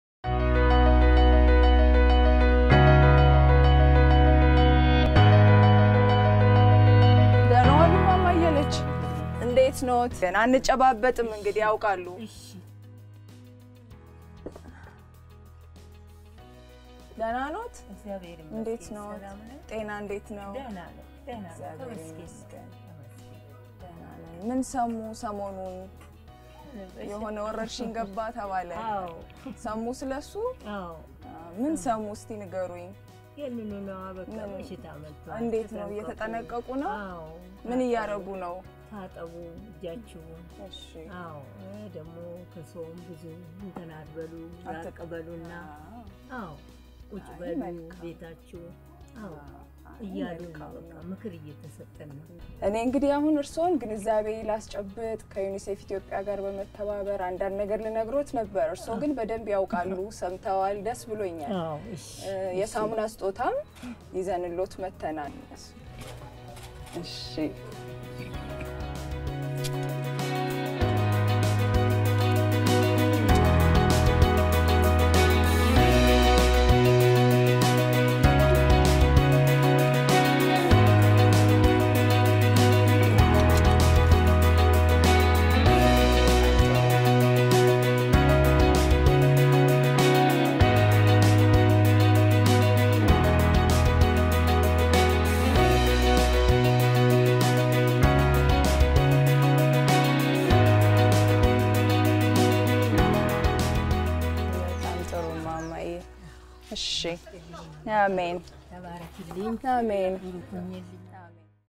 Zeritu Kebede visits families to demonstrate how to prevent
Together, we can stop the spread of Coronavirus!

UNICEFEthiopia supporter ZerituKebede visited families at Yeka Subcity to help distribute soap to underprivileged families and demonstrate how to wash hands and prevent coronavirus.

UNICEF is distributing 60, 000 bars of soap to underprivileged families in AddisAbaba.